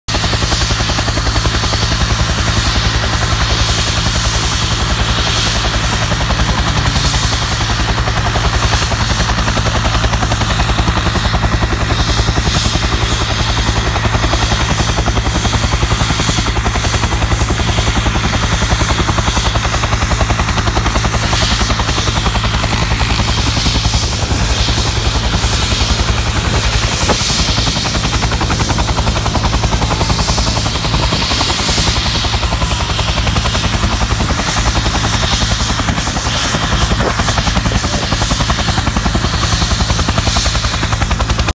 Incendio Cornatel / Infobierzo
Incendio que llegó a nivel 2 en Borrenes y Cornatel